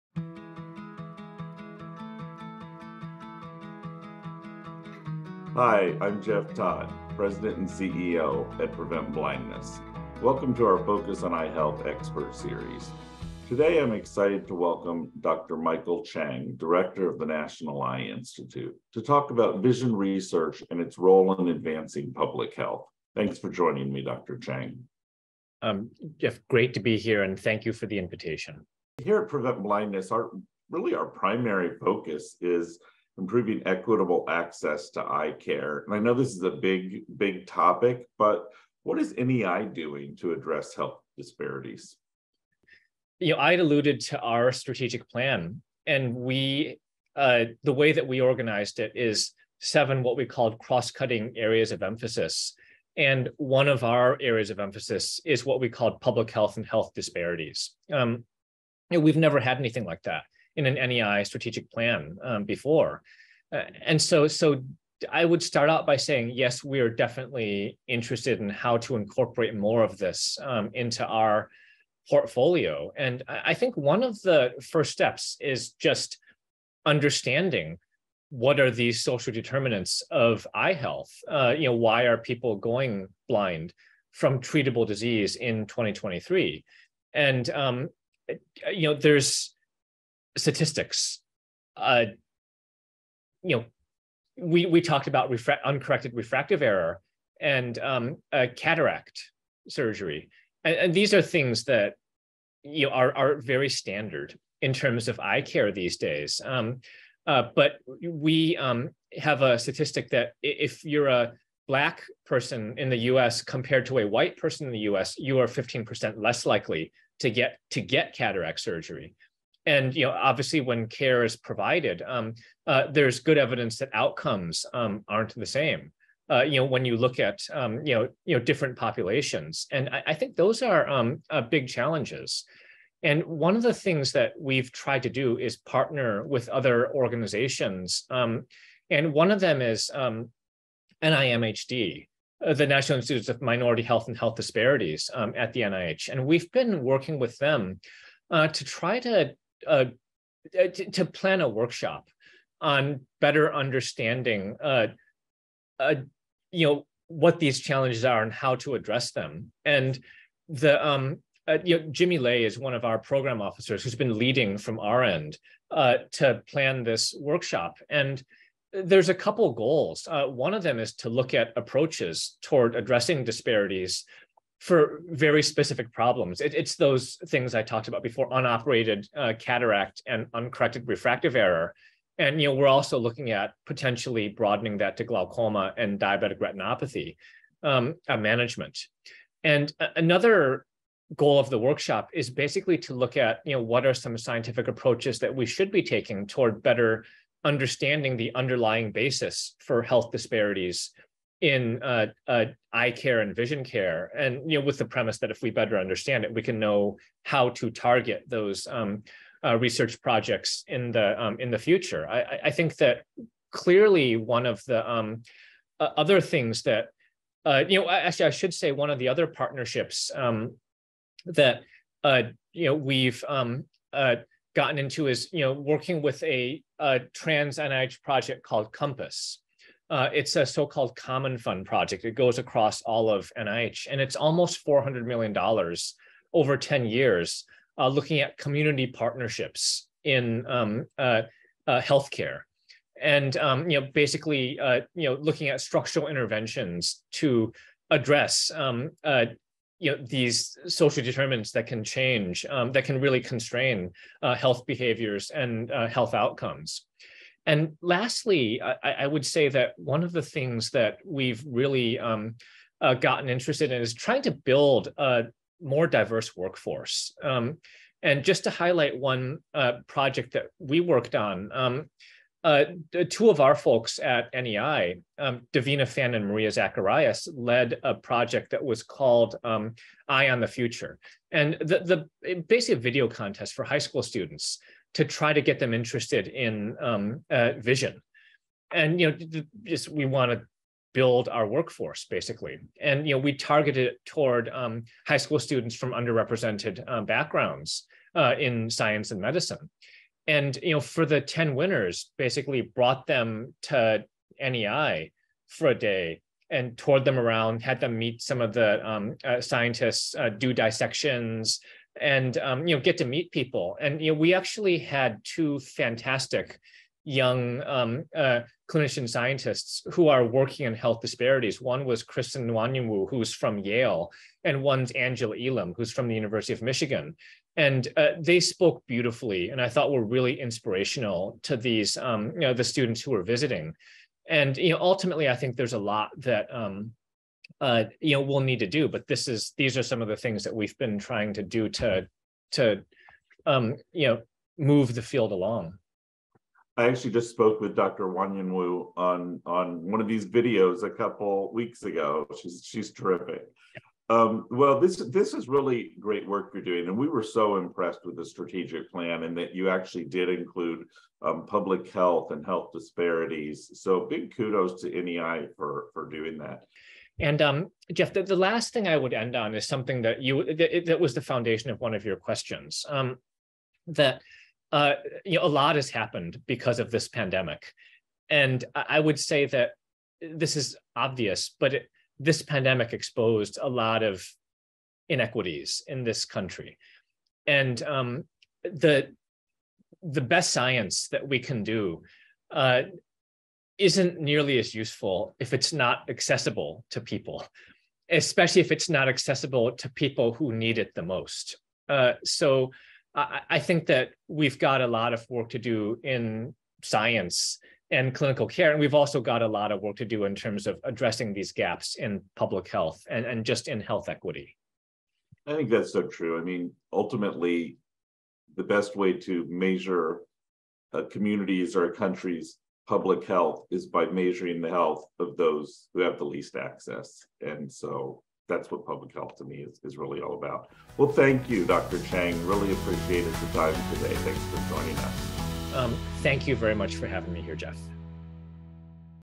Health Equity and Access to Care - An Interview with Dr. Michael F. Chiang
Prevent Blindness president and CEO, Jeff Todd, discusses health equity, access to care, and the work of the National Eye Institute with Michael F. Chiang, MD, Director of the National Eye Institute. Find out more about the Focus on Eye Health Expert Series